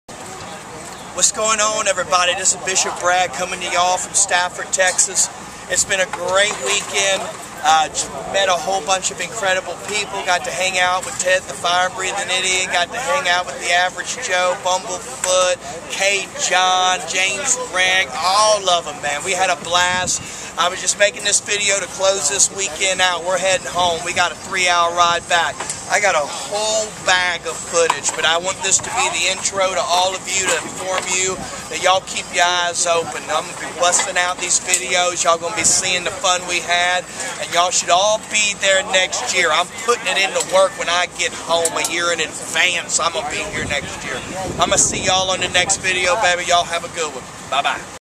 The FINAL vid of the Houston Hot Sauce Festival 2013/ Bishop Brads Pepper TV
Festival was in credible. Hope you all enjoyed the footage! Till next year...hope to see ya there.